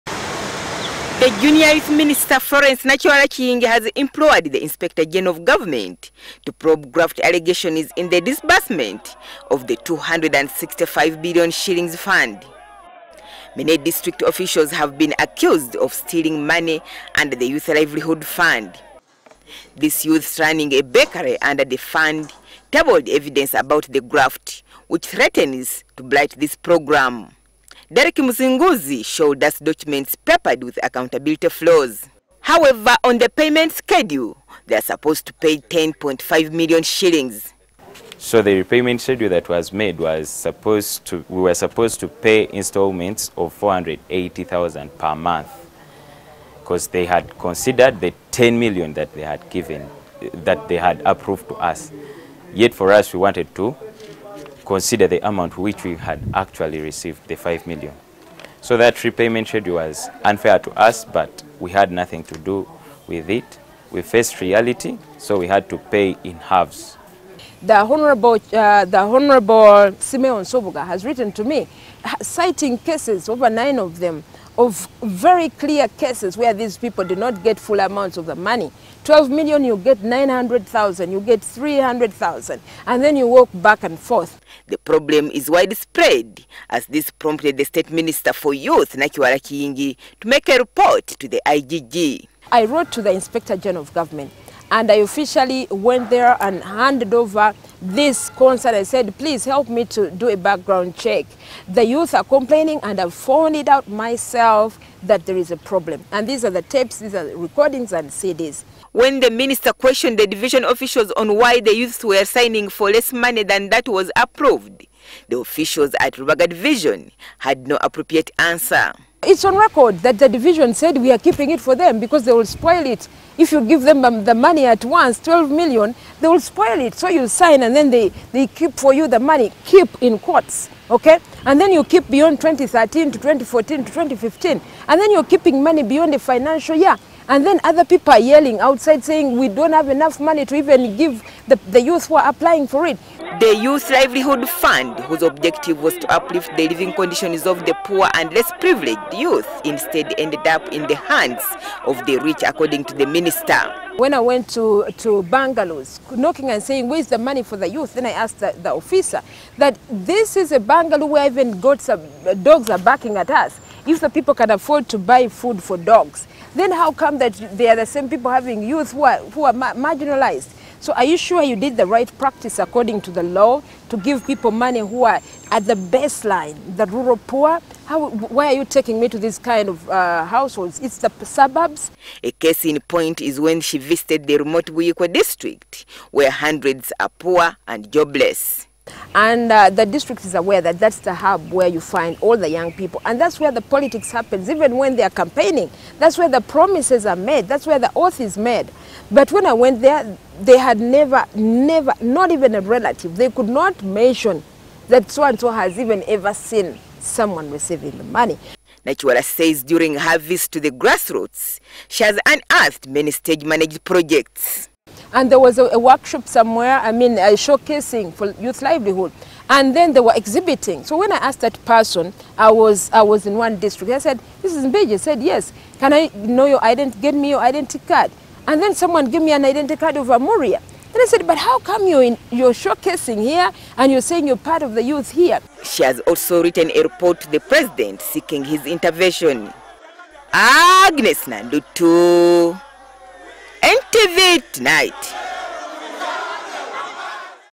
Youth minister Florence Nakiwala Kiyingi petitions IGG to investigate graft in youth livelihood fund
The State Minister for Youth Nakiwala Kiyingi has petitioned the Inspect General of Government to probe allegations of graft into the 265 billion Youth Livelihood Fund.This was after she unearthed a litany of accountability flaws in the programme meant to help less privileged youths.As Agness Nandutu reports, she has also implored President Yoweri Museveni to intervene.